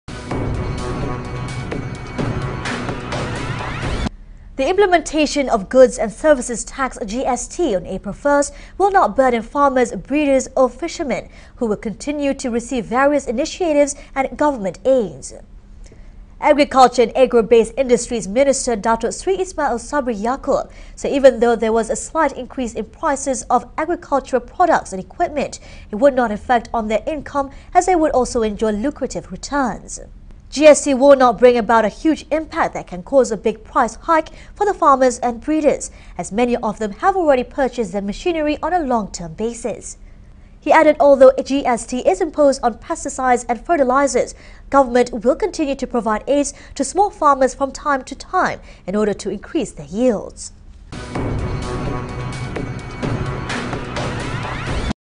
GST Malaysia Will Not Burden Farmers, Breeders, Fishermen
The implementation of Goods and Services Tax (G-S-T) on April 1st will not burden farmers, breeders or fishermen, who will continue to receive various initiatives and government aids.